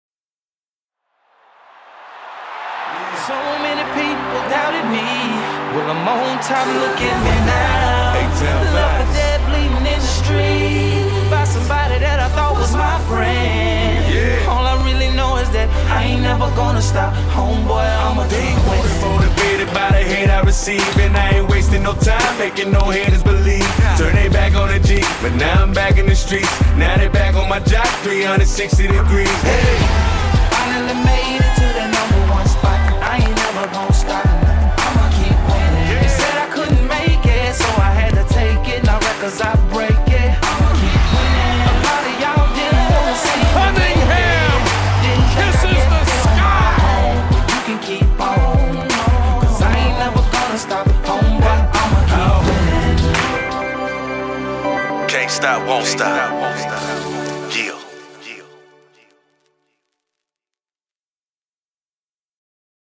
2011-12 OSU MBKB Intro Video
Check out the new Intro Video for the 2011-12 Oregon State men's basketball team! This video is shown on the video boards in Gill Coliseum prior to the men's basketball team being introduced before tip-off.